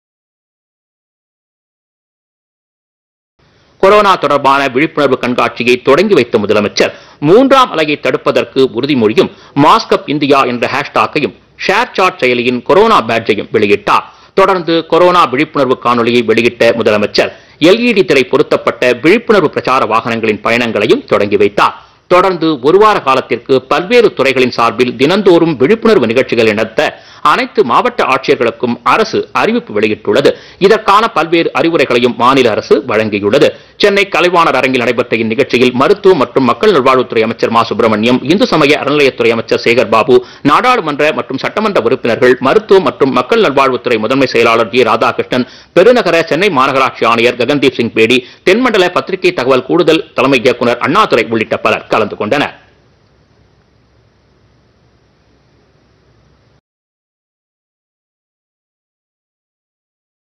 கொரோனா விழிப்புணர்வு பிரச்சாரம்
கொரோனா விழிப்புணர்வு பிரச்சார வாகனத்தை தொடங்கி வைத்தார்  தமிழக முதலமைச்சர் மு க ஸ்டாலின்.